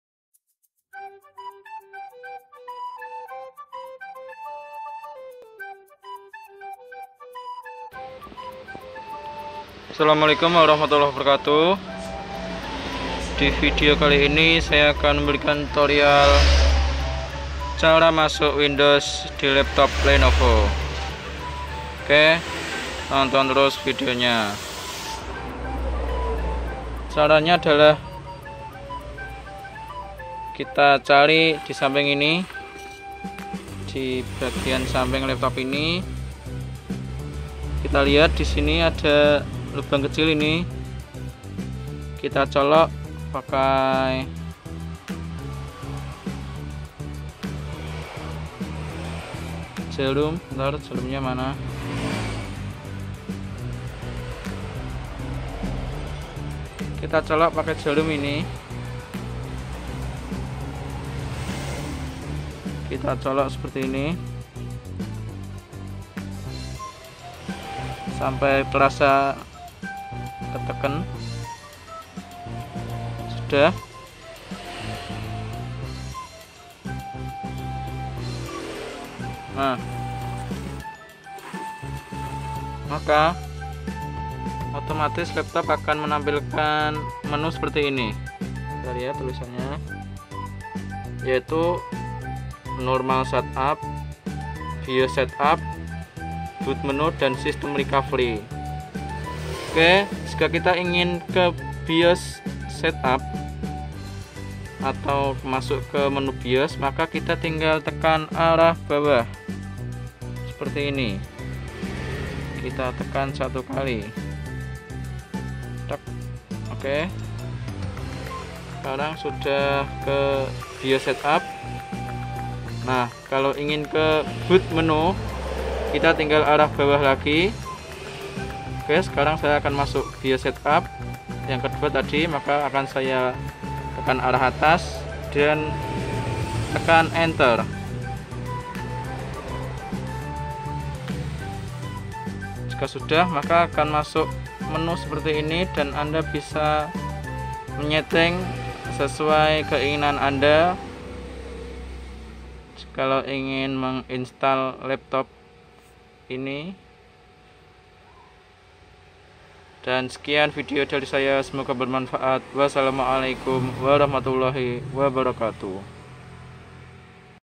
CARA MASUK BIOS LAPTOP LENOVO TERBARU
untuk menginstall windows biasanya kita harus masuk bios / boot menu terlebih dahulu, di laptop lenovo ada tombol kecil yang bila di colok dengan jarum / peniti maka akan masuk menu bios / boot menu option. untuk lebuh jelasnya tonton video ini.
Bizmaunik computer
Jl. Surabaya No. 498 Dengok  -Kec. Padangan  Kab. Bojonegoro